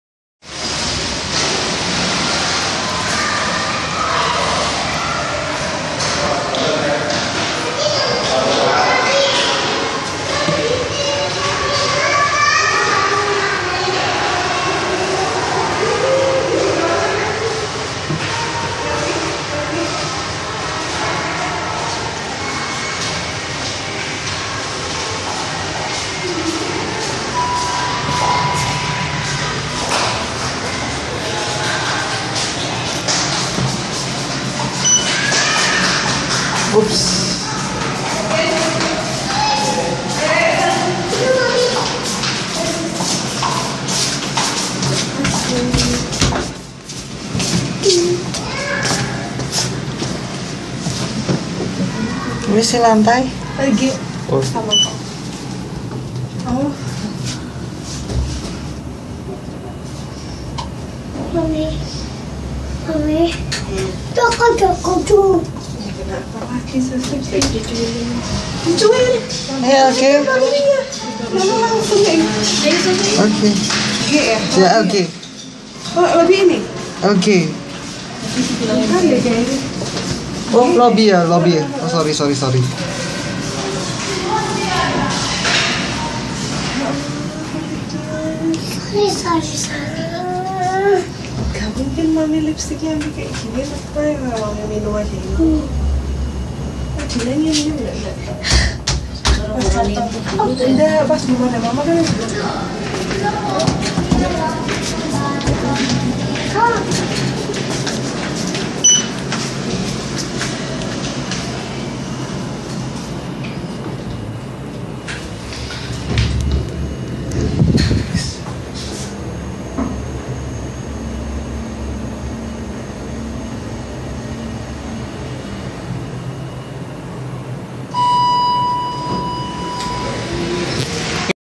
Nice Kone Elevator at Golden Tulip Holland Resort, Batu (Terrace)
Same with the main elevator ones

Year Installed : ?
Type : N Monospace, KDS 50 Black Screen, Round AUS Buttons
Floor Serve : L1, LG, G
Capacity : 13 Pers., 1000 Kg
Camera : Samsung Grand 2